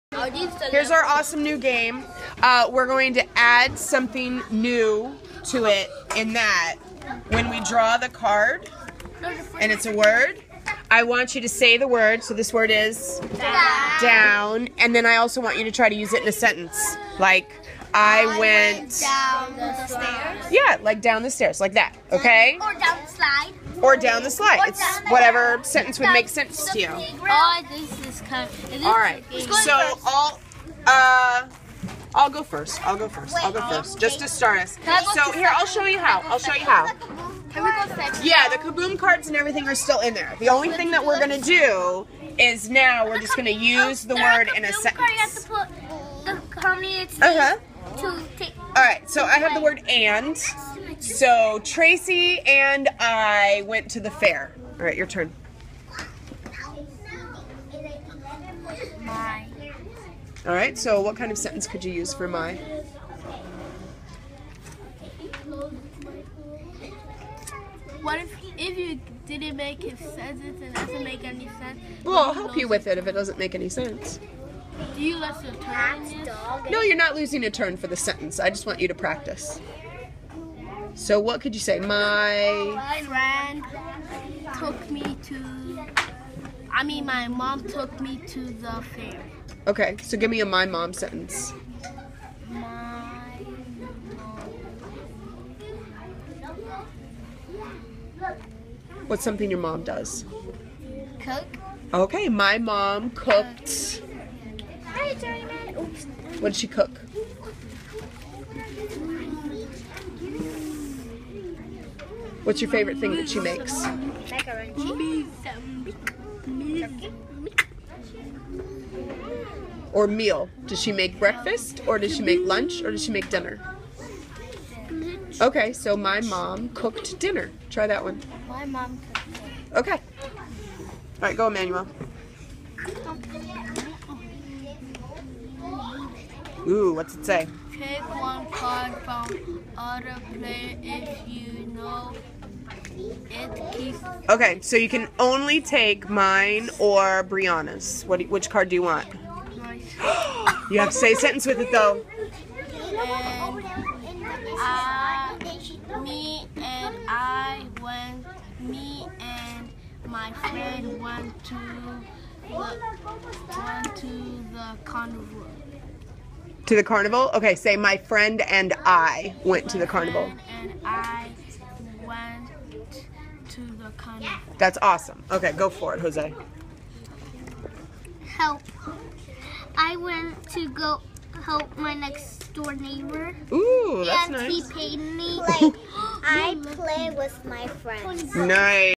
Superhero Smackdown with Sentence Builder
I added on the variation of having to create a sentence with the sight word the student drew for a couple of reasons.  The first was to help my Ells with some of their oral language goals.  The second reason was that it helped me create a situation in which the students provided daily teachable moments for the rules of English grammar without having to present it in a worksheet.  In the beginning, the kids came up with very basic sentences, but as they became more comfortable, I was able to coax them into creating more detailed sentences.  This has started to slowly encroach into their writing as well.